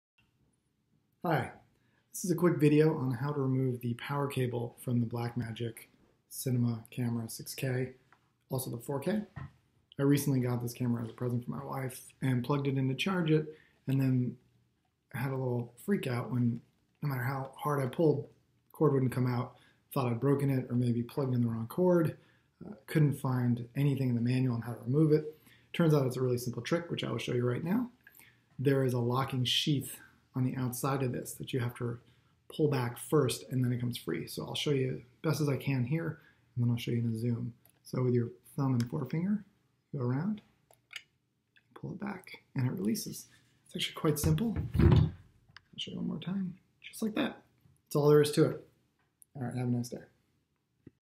How to remove power cable from Blackmagic pocket cinema camera
If you're struggling to remove the power cable from your Blackmagic pocket cinema camera, here's what you might be missing.